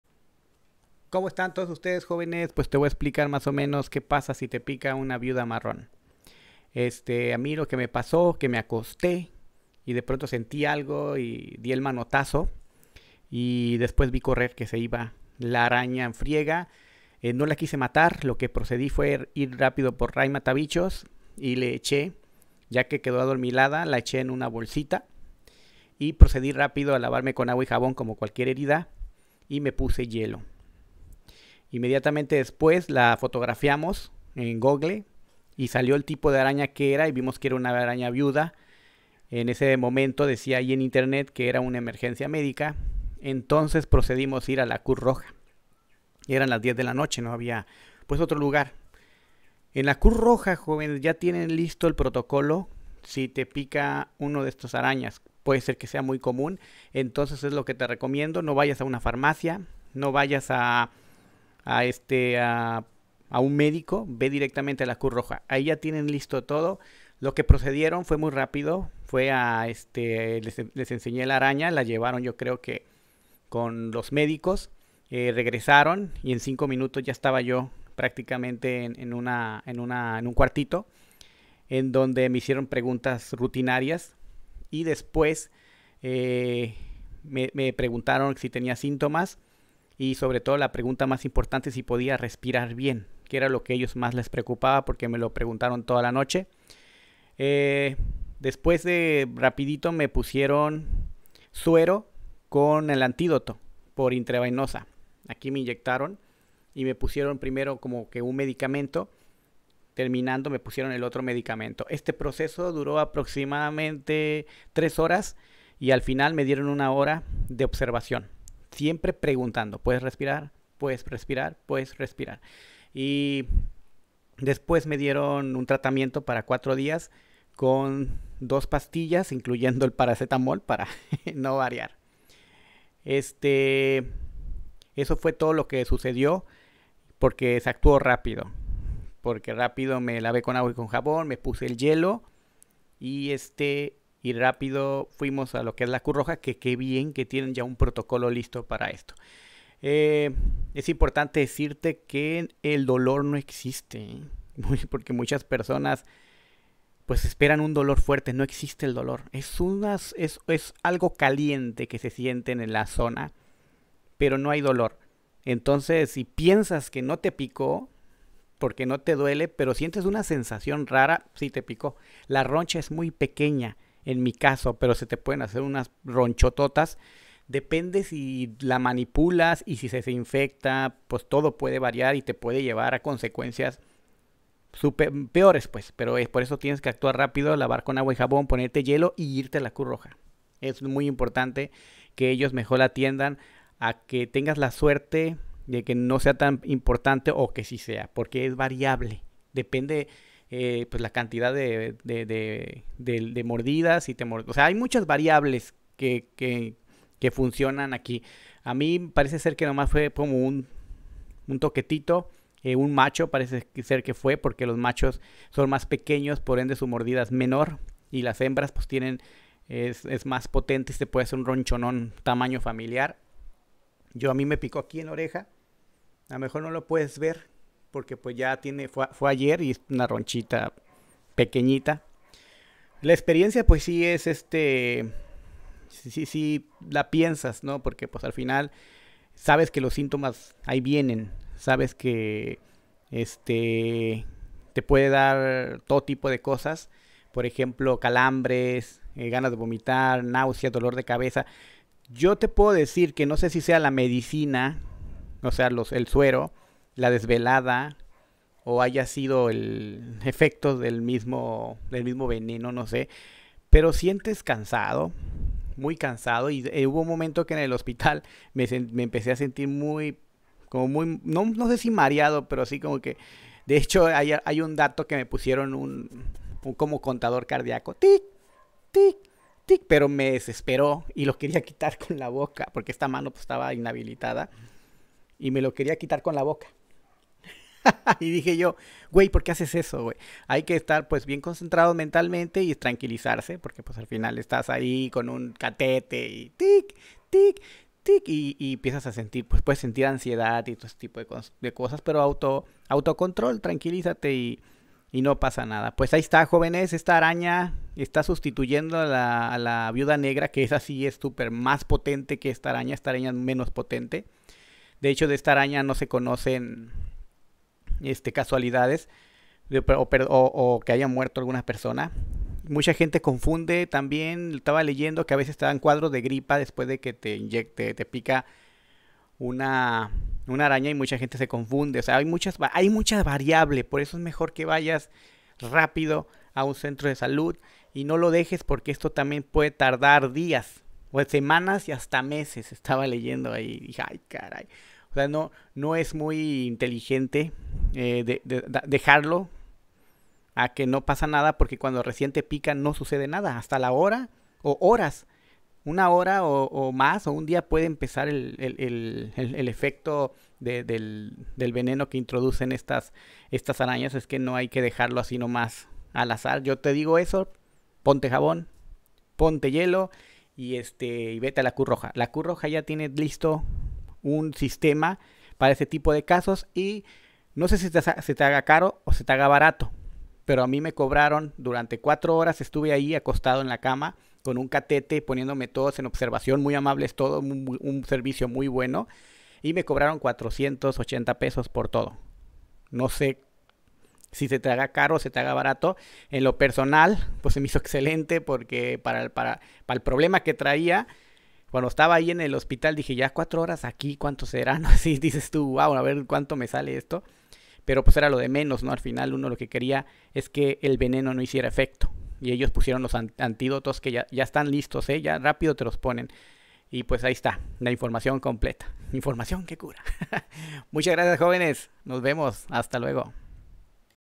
VIUDA MARRON ME MORDIO COMO REACCIONE PARA EVITAR PROBLEMAS
me mordio una araña viuda marron, reacciones rápido para evitar complicaciones